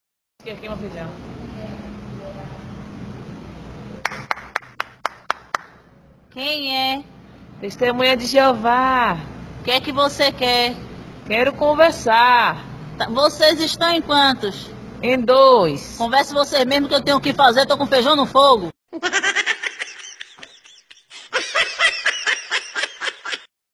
Piada das Testemunhas de Jeová
Uma Piada Hilária Sobre A Visita De Duas Testemunhas de Jeová A Uma Residência Em Uma Manhã de Segunda-feira...